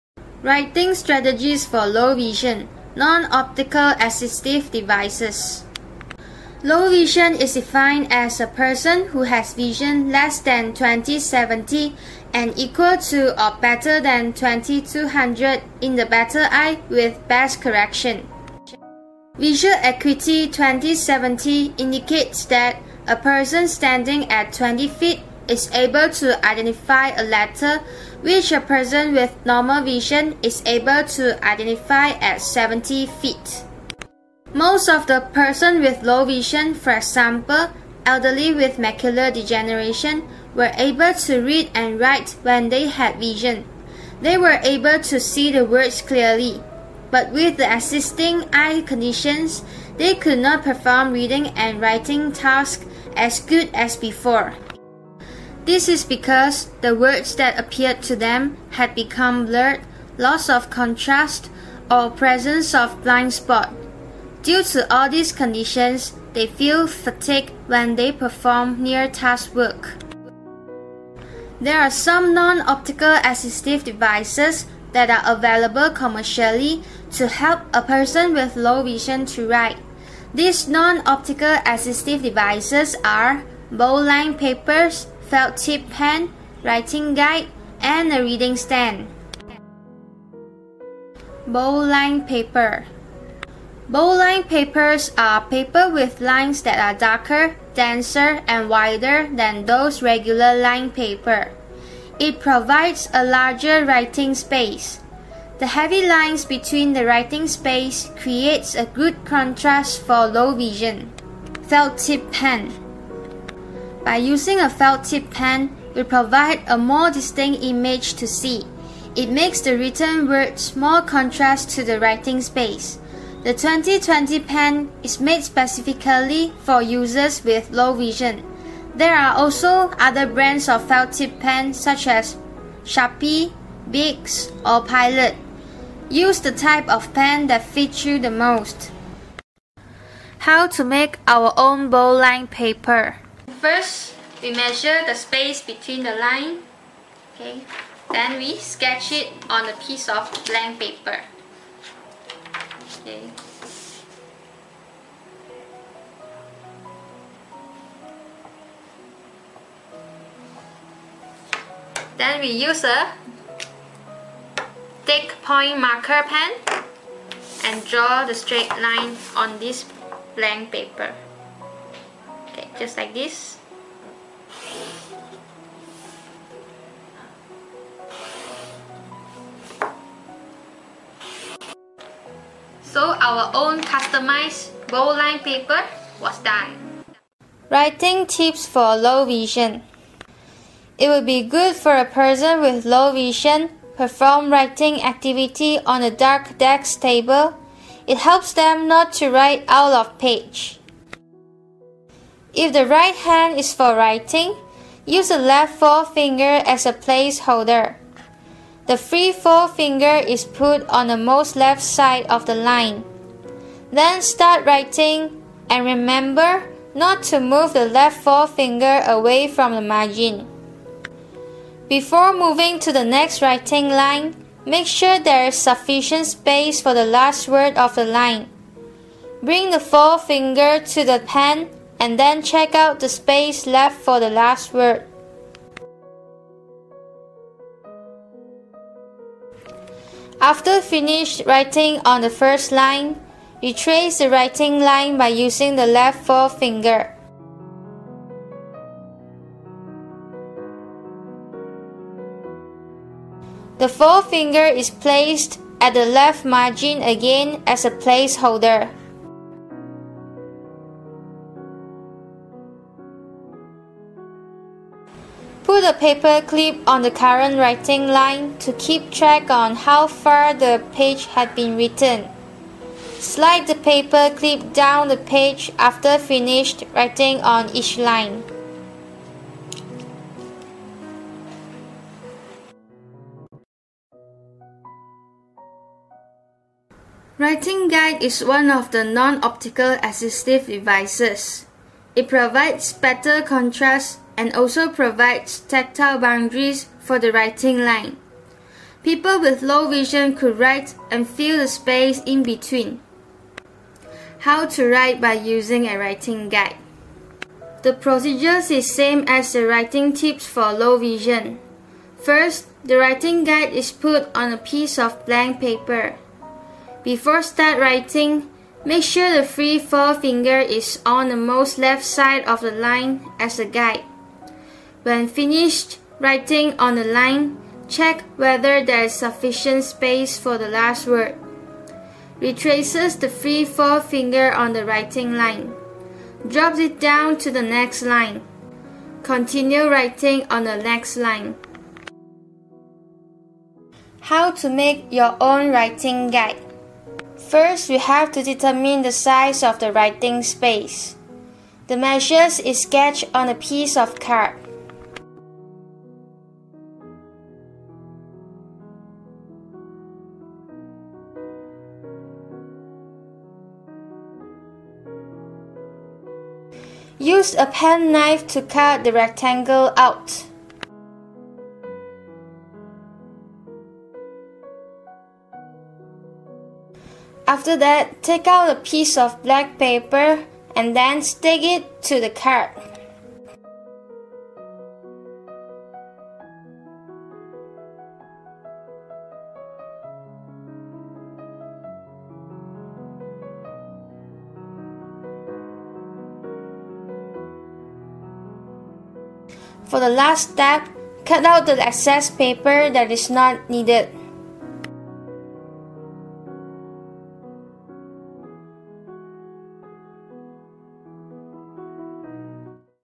Writing Strategies for Low Vision (Non-optical devices)
A person with Low vision such as an adult with macular degenegration has problem in near task work such as reading and writing..
Non-optical devices such as a bold line papers, felt-tip pen, writing guide and a reading stand.
This video showed you how to make some of the non-optical devices.